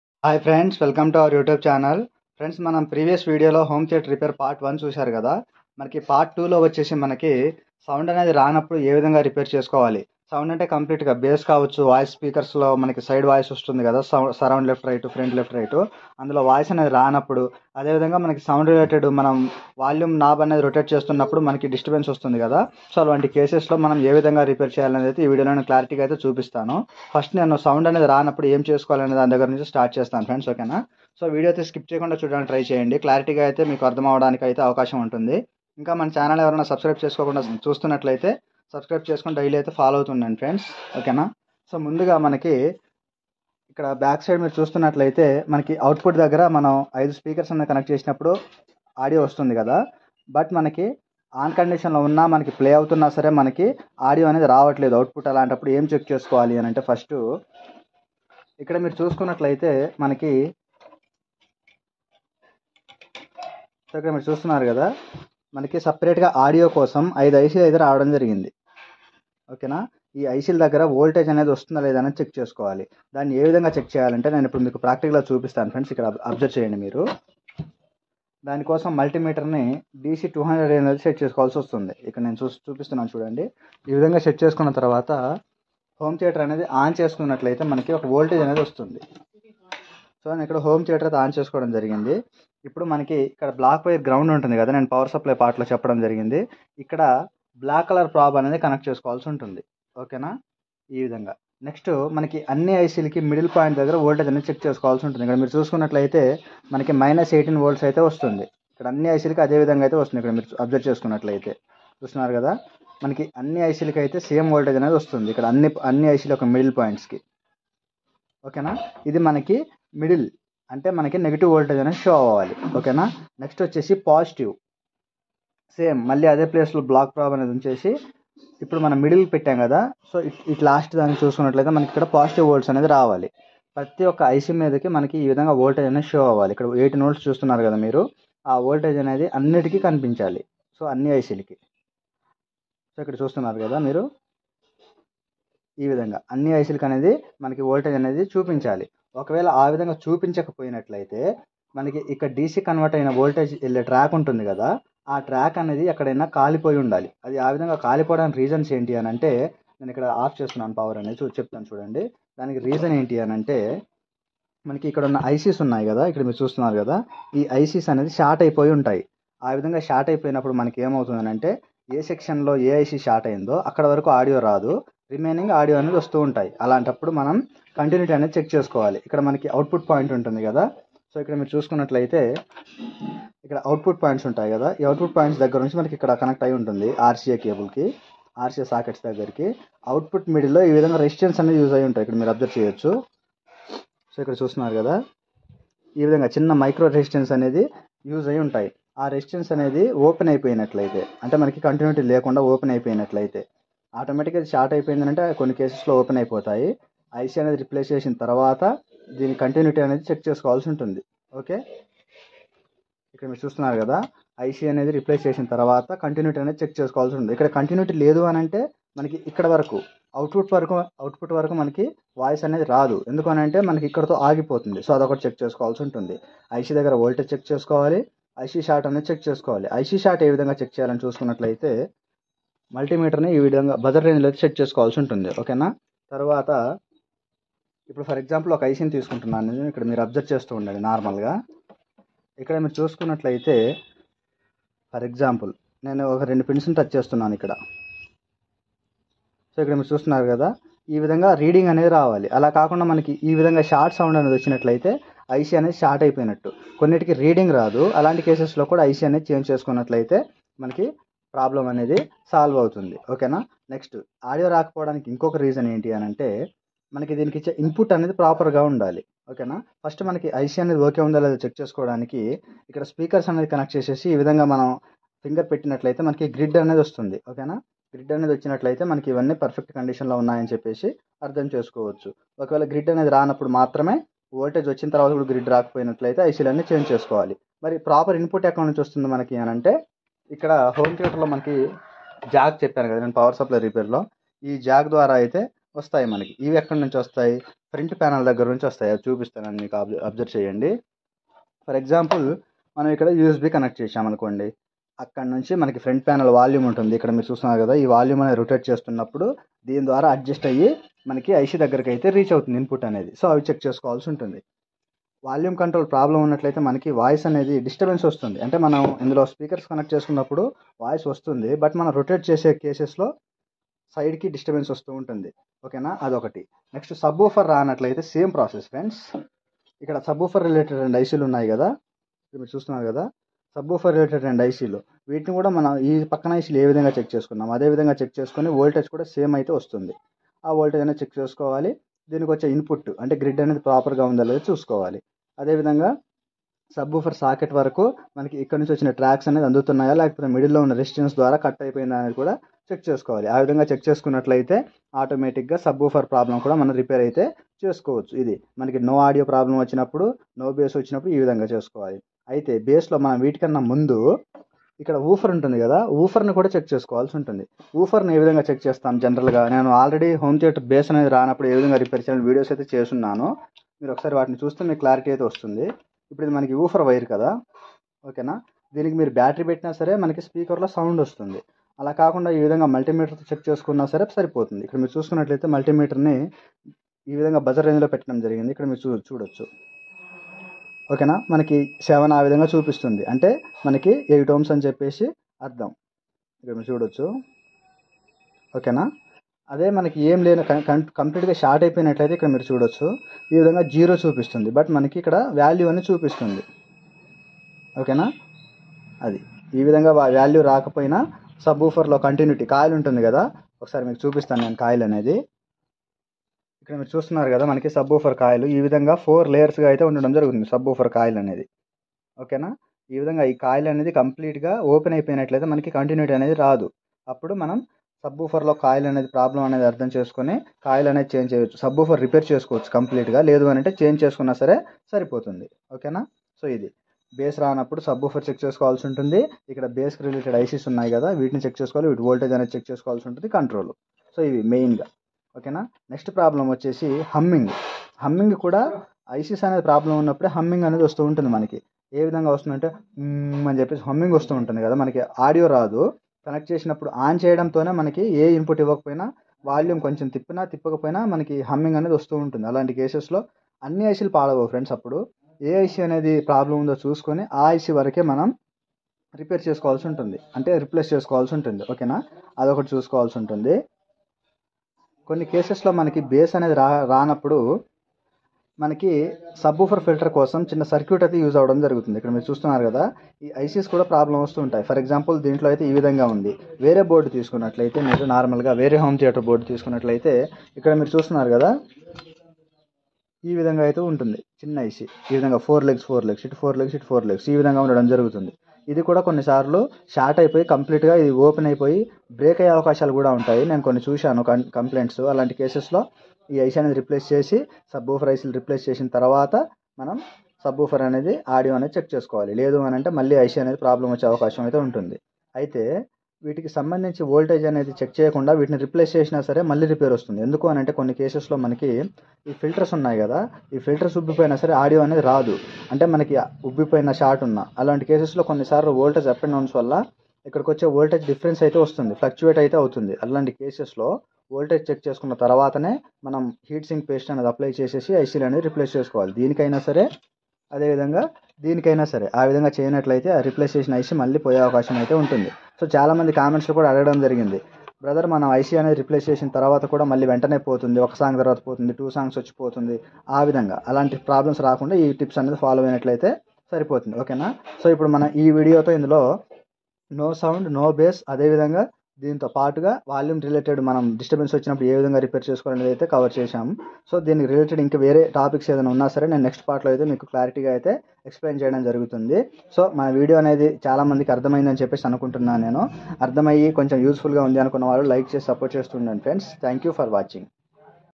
how to repair home theatre / no bass / no sound / part 2 / in telugu
home theatre full repair course part 2...
  in this video home theatre no sound and bass problems repair in telugu..